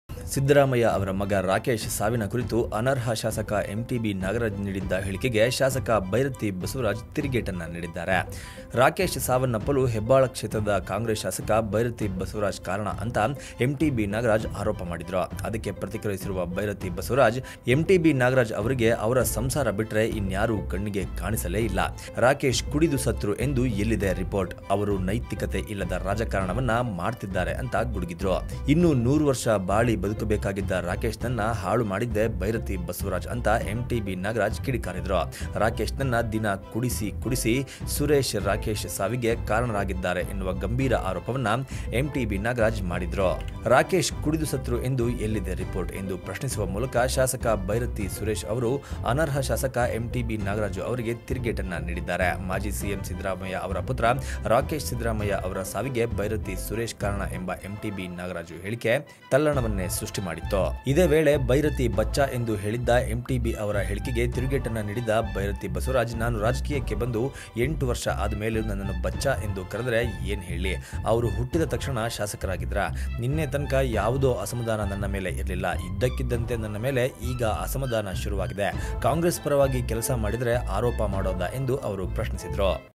ರಾಕೇಶ್ ಸಾವಿನ ರಹಸ್ಯ ಹೇಳಿದ MTB..! | MTB Nagaraj | Oneindia Kannada
ಸಿದ್ದರಾಮಯ್ಯ ಮಗನ ಸಾವಿನ ಕುರಿತು ಅನರ್ಹ ಶಾಸಕ ಎಂಟಿಬಿ ನಾಗರಾಜ್ ನೀಡಿದ್ದ ಹೇಳಿಕೆಗೆ ಶಾಸಕ ಬೈರತಿ ಬಸವರಾಜ್ ತಿರುಗೇಟು ನೀಡಿದ್ದಾರೆ. ರಾಕೇಶ್ ಸಾವನ್ನಪ್ಪಲು ಹೆಬ್ಬಾಳ ಕ್ಷೇತ್ರದ ಕಾಂಗ್ರೆಸ್ ಶಾಸಕ ಭೈರತಿ ಸುರೇಶ್ ಕಾರಣ ಎಂದು ಎಂಟಿಬಿ ನಾಗರಾಜ್ ಆರೋಪಿಸಿದ್ದರು. ಅದಕ್ಕೆ ಪ್ರತಿಕ್ರಿಯಿಸಿರುವ ಭೈರತಿ ಎಂಟಿಬಿ ನಾಗರಾಜ್ ಅವರಿಗೆ ಅವರ ಸಂಸಾರ ಬಿಟ್ಟರೆ, ಇನ್ಯಾರೂ ಕಣ್ಣಿಗೆ ಕಾಣಿಸಲ್ಲ. ರಾಕೇಶ್, ಕುಡಿದು ಸತ್ರು ಎಂದು ಎಲ್ಲಿದೆ ರಿಪೋರ್ಟ್? ಅವರು ನೈತಿಕತೆ ಇಲ್ಲದ ರಾಜಕಾರಣ ಮಾಡುತ್ತಿದ್ದಾರೆ ಎಂದು ಗುಡುಗಿದ್ದಾರೆ.

 Byrathi Basavaraj has issued a statement on the Disqualified MLA MTB Nagaraj's statement on Siddaramaiah's son's death
Oneindia kannada